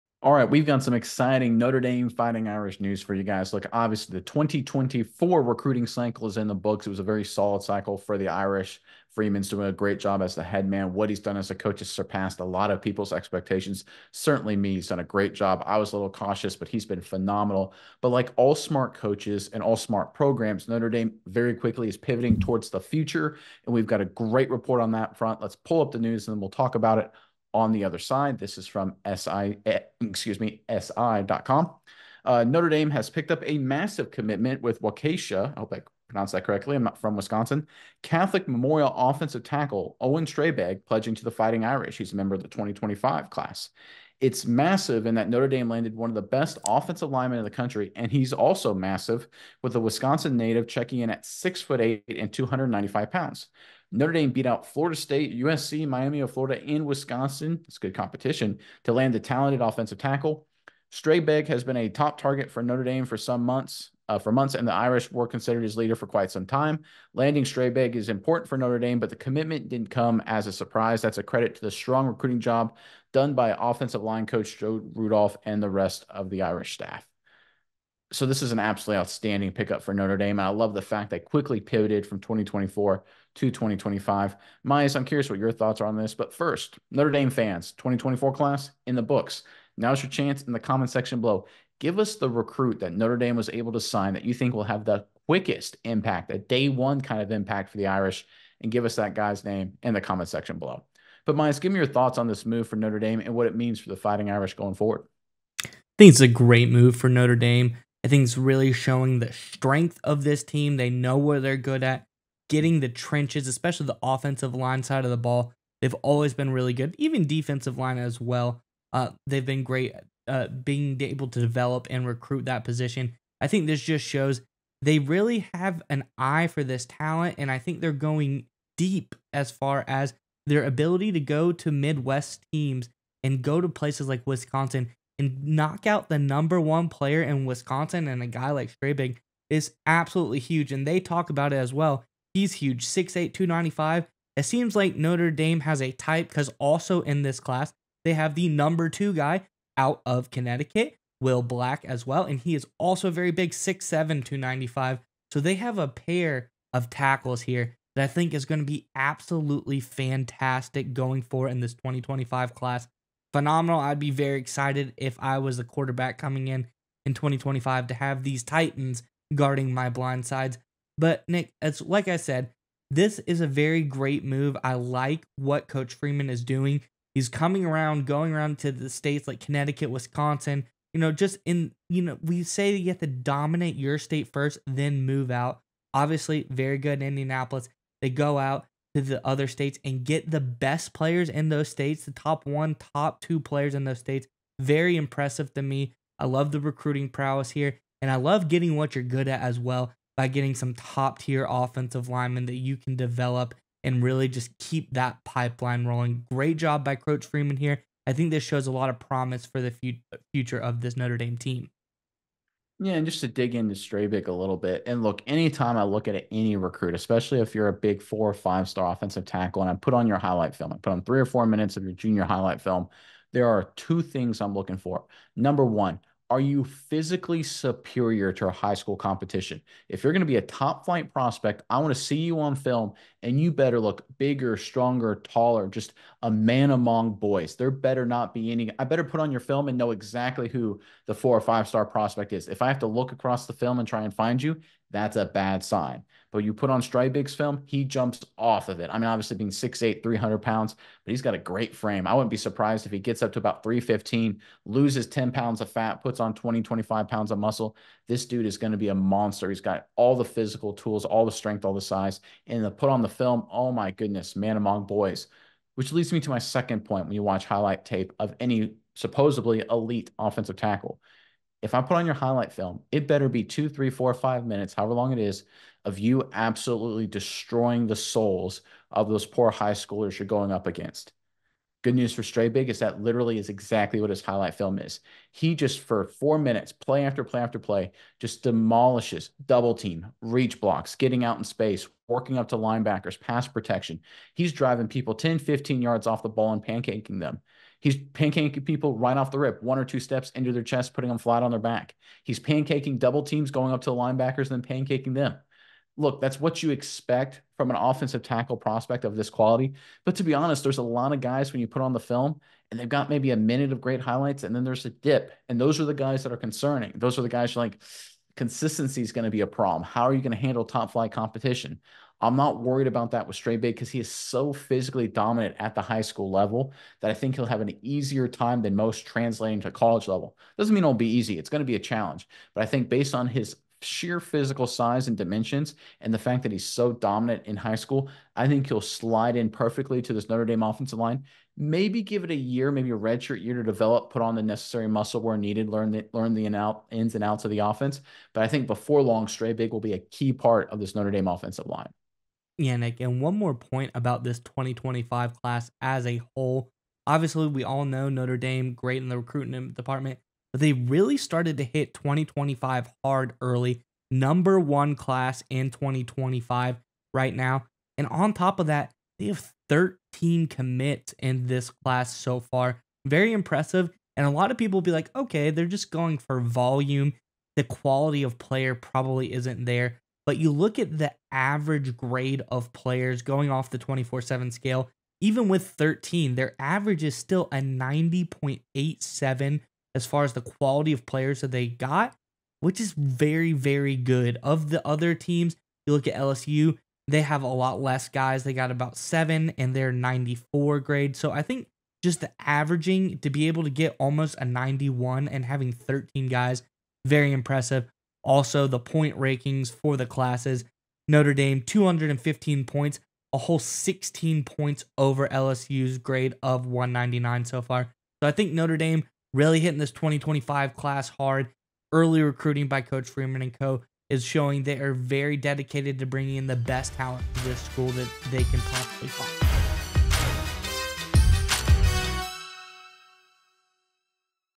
Notre Dame Fighting Irish Just Pulled Off A SNEAKY GOOD Move
The Notre Dame Fighting Irish football program has just wrapped up its most recent recruiting class.But like all great programs, Notre Dame is already looking towards the future. To that end, Notre Dame has secured a commitment of one of the top prospects in the country. Fans of the Notre Dame Fighting Irish should be excited by this news.